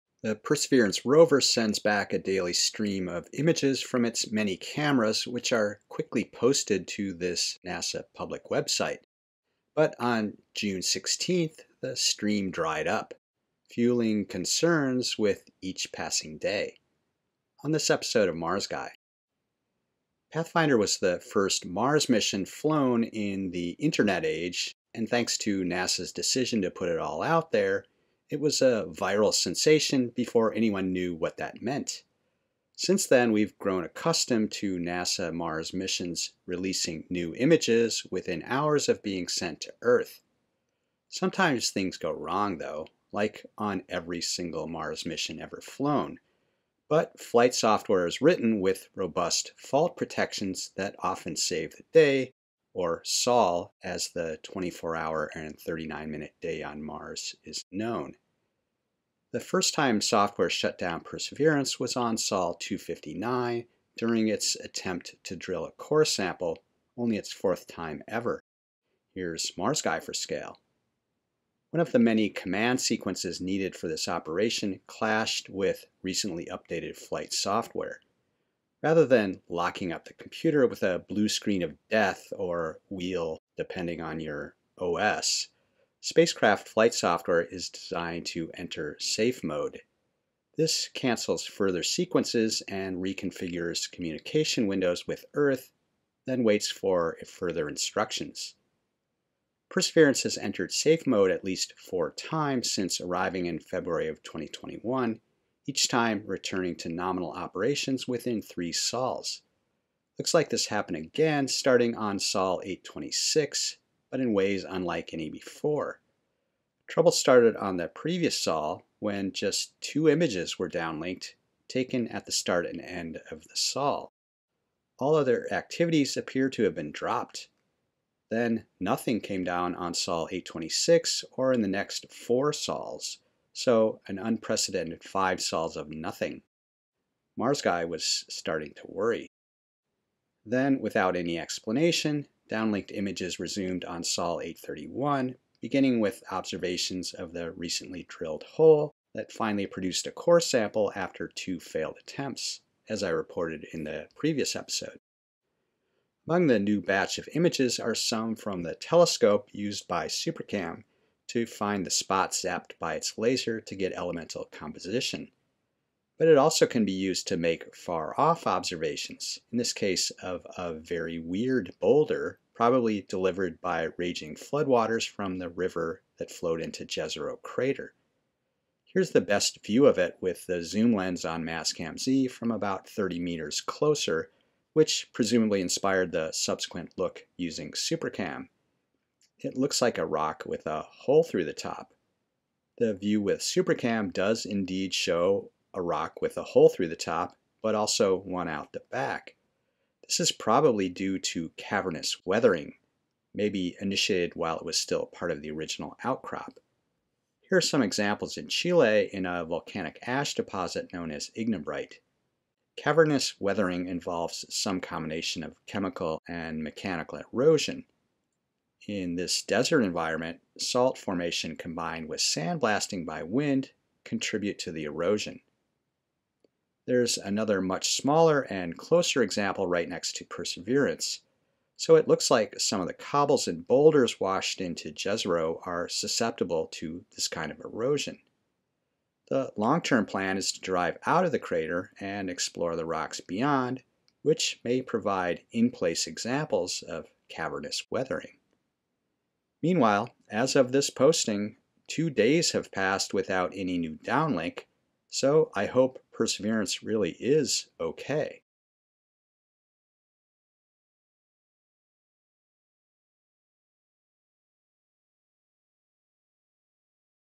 Five days without downlink from Mars
Episode 116
The Perseverance rover sends back a daily stream of images from its many cameras, which are quickly posted to a NASA public website.  But on June 16th, the stream dried up, fueling concerns with each passing day.